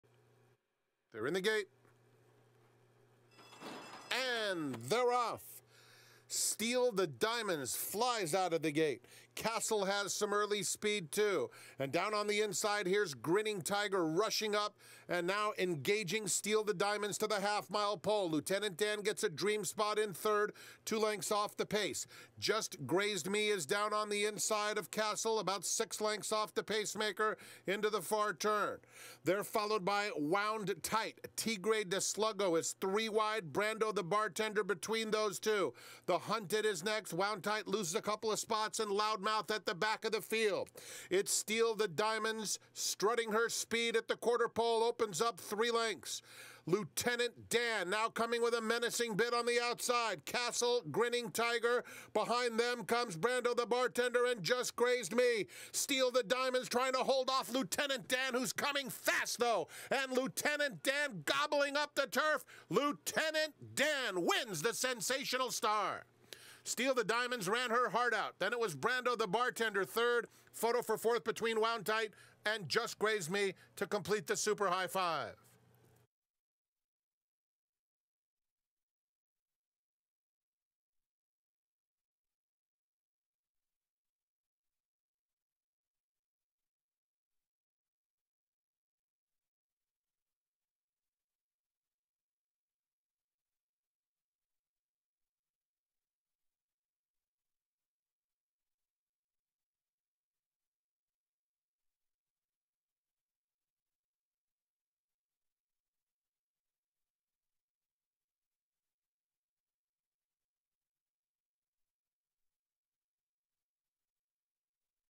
Sensational Star Stakes - March 22, 2020
Lieutenant Dan wins the Sensational Star Stakes